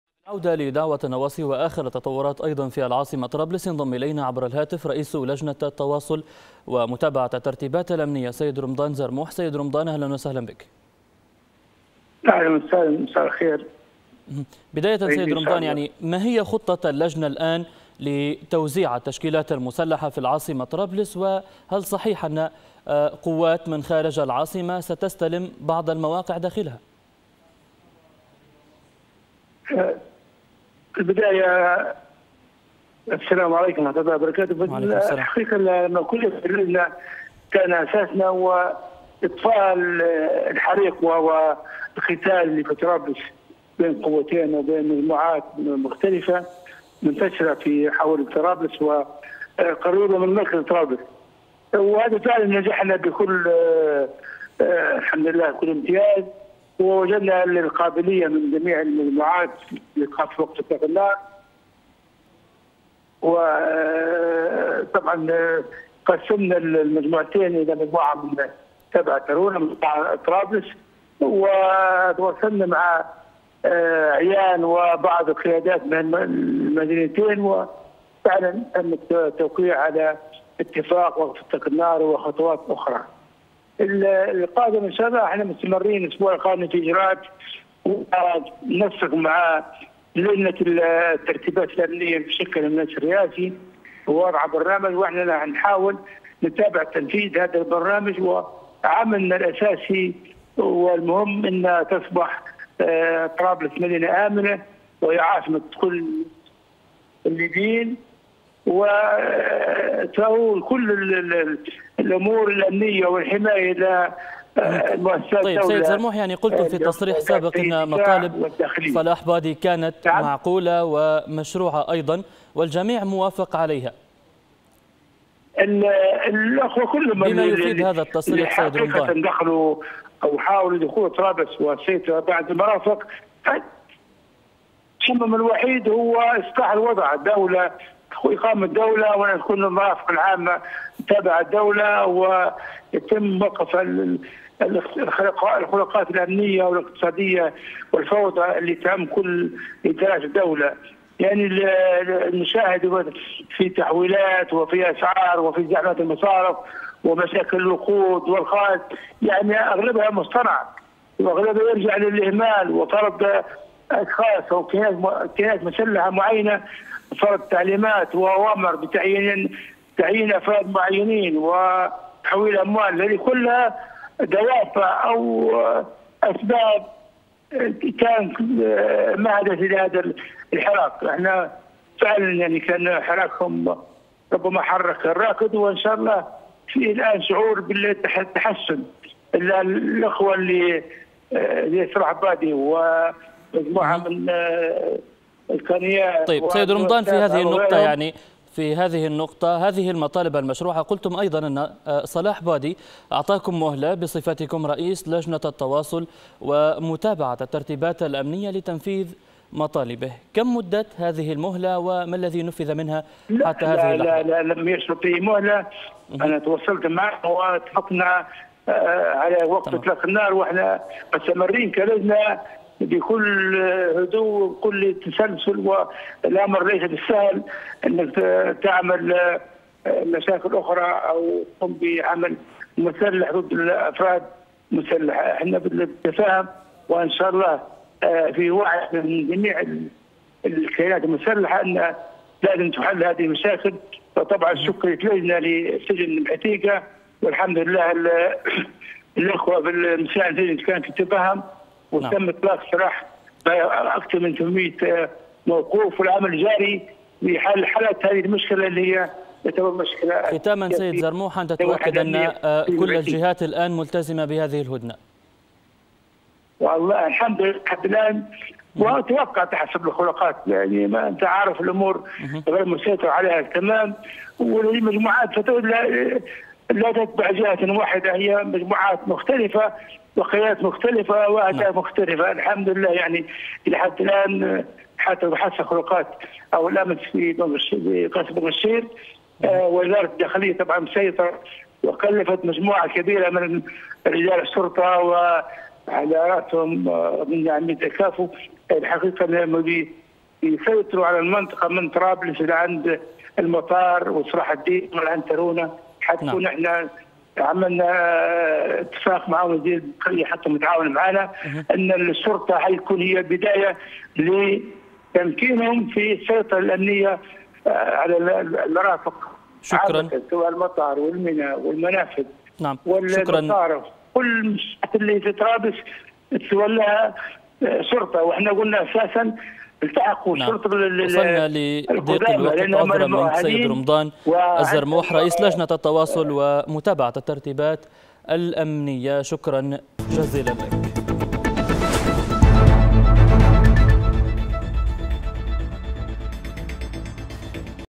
رمضان زرموح: كل الأطراف ملتزمة باتفاق وقف الاشتباكات في طرابلس ونعمل على تنفيذ بقية الترتيبات
رئيس لجنة الترتيبات الأمنية بداخلية المقترحة رمضان زرموح: كل الأطراف ملتزمة باتفاق وقف الاشتباكات في طرابلس ونعمل على تنفيذ بقية الترتيبات